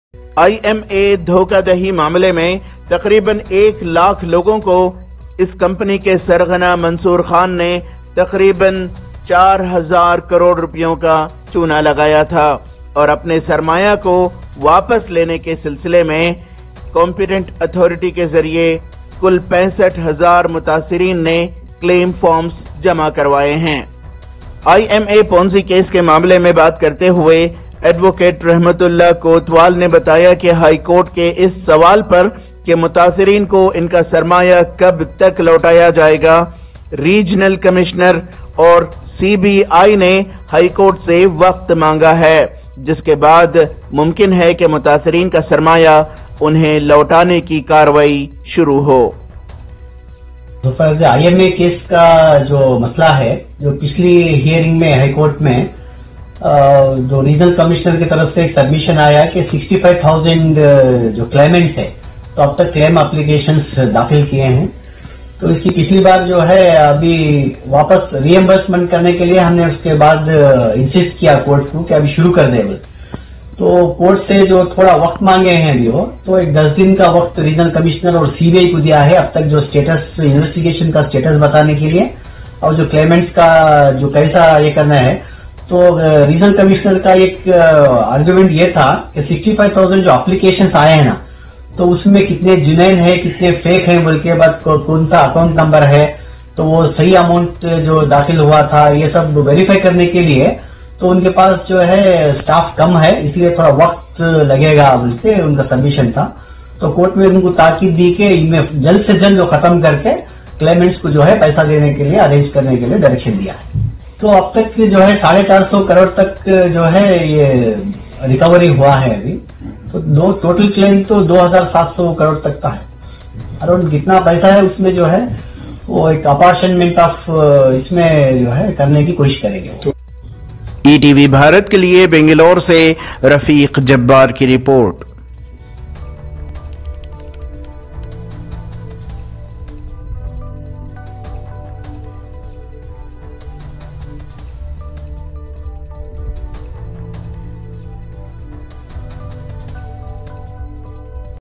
IMA PONZI SCAM: HOPE FOR REIMBURSEMENT OF INVESTMENT TO THE VICTIMS FROM HC
*IMA PONZI CASE MEIN MUTASSIREEN KO SARMAYA MILNE KI UMEED, ADV. REHMATULLAH KOTWAL* 🛡️

*ETV 🅱️ URDU REPORT*